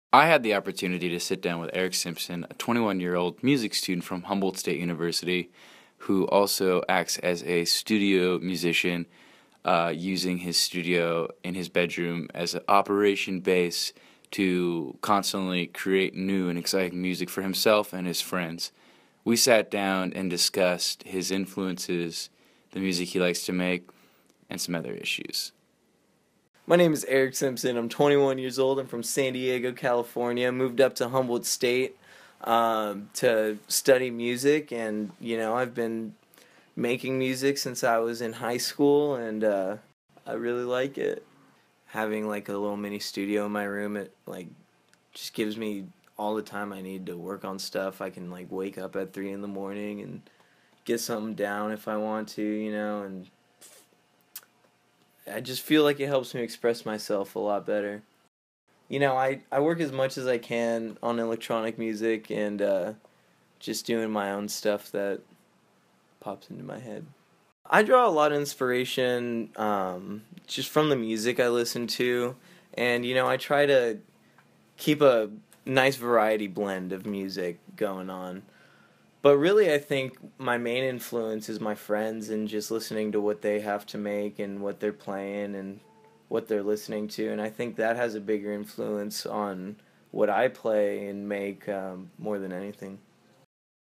Eric Simpson says a few things
I asked Eric about his adventures in home recording for a class project.